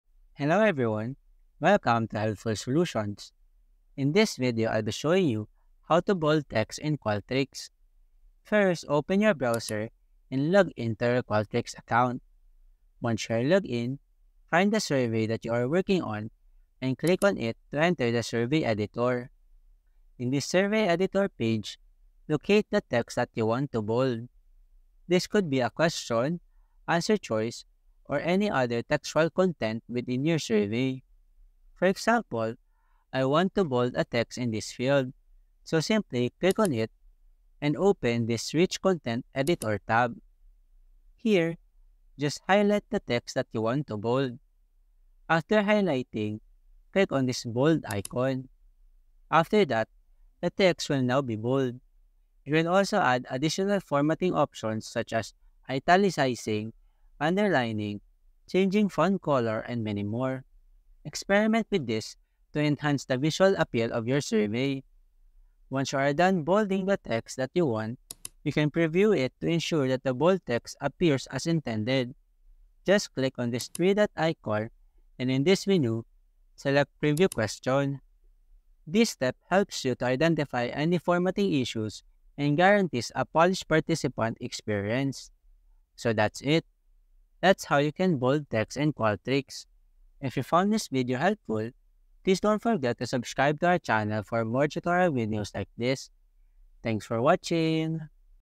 How To Bold Text In Qualtrics (How To Format Text In Qualtrics)
How To Bold Text In Qualtrics (How To Format Text In Qualtrics). In this video tutorial I will show how to Bold text in Qualtrics.